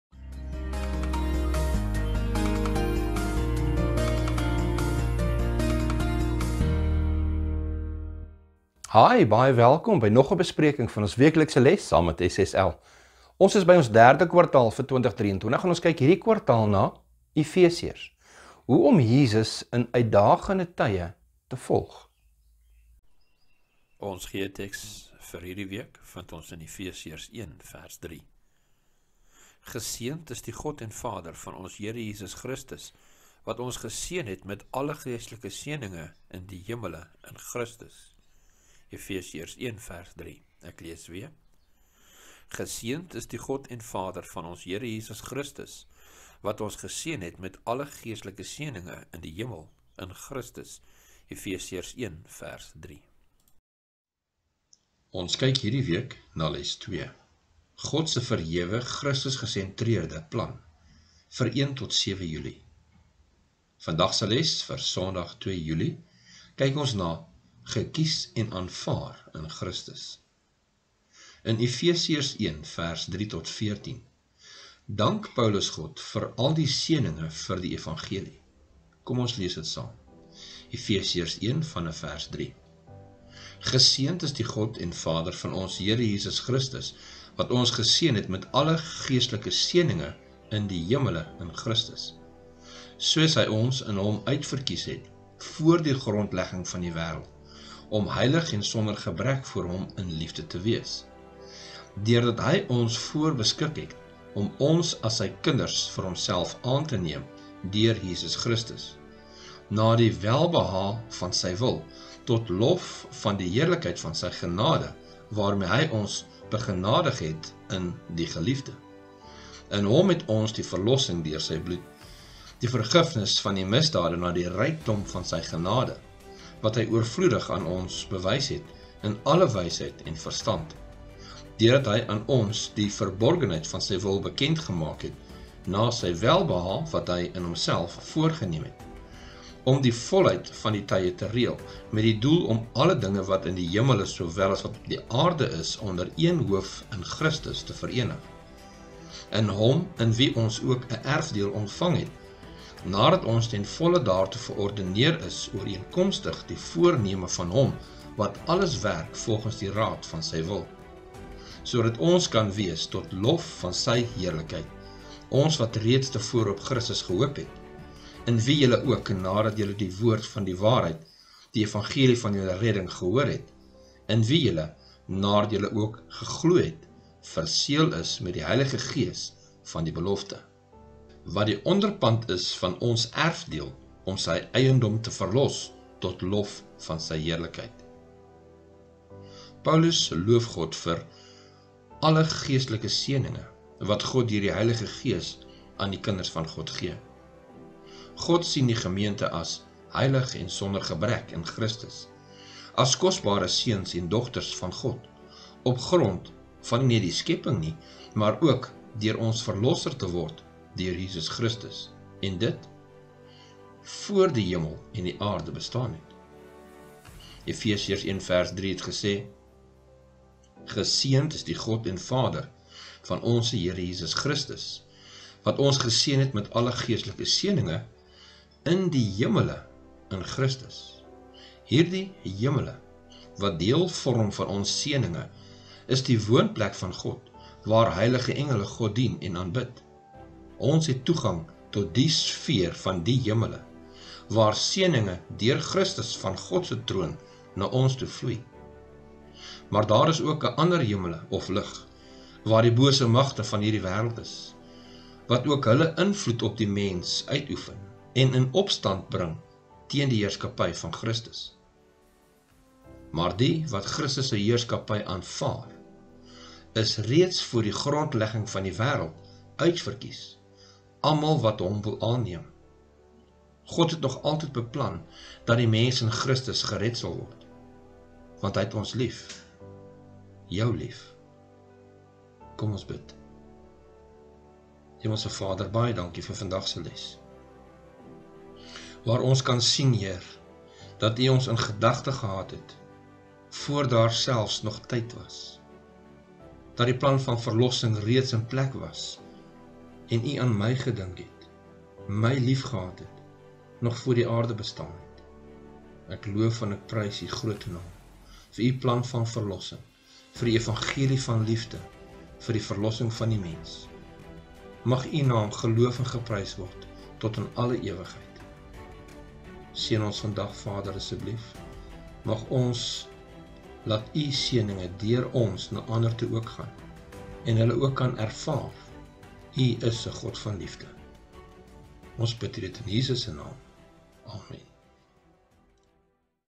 Sonsdag 02 Julie - Gekies en aanvaar in Christus
Les 2 – God se verhewe Christus gesentreerde plan

Aanbieder:
David Kühn
CREDIT:

Jesus Christus.